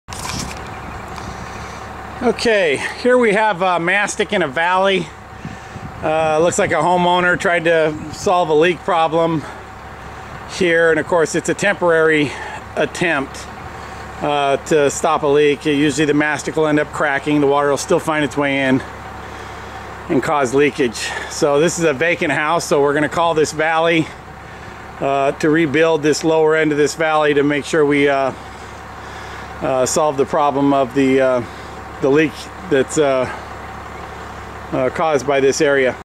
Roof Doctors | Roof Repairs Florin California
Adam Wilfley | Roof Doctors | describes a bad leak repair that was done on a composition roof valley on a home in Florin California .   Please contact our office for a free roof inspection or a free roof estimate.